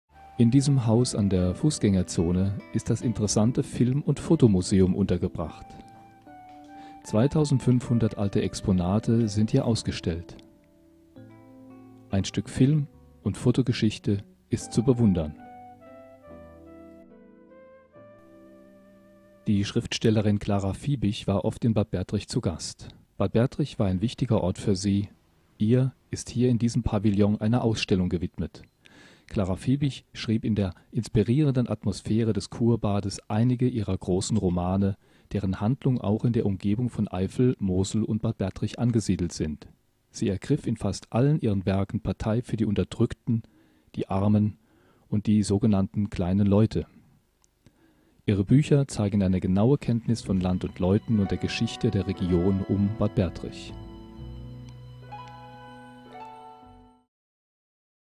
Bad Bertrich - Filmmuseum und Clara Viebig
Das Foto- und Filmmuseum in Bad Bertrich zeigt seltene Exponate aus der Welt der Fotografie und der Geschichte des Films. Clara Viebig machte Bad Bertrich berühmt in ihren heimatlichen Erzählungen aus der Eifel.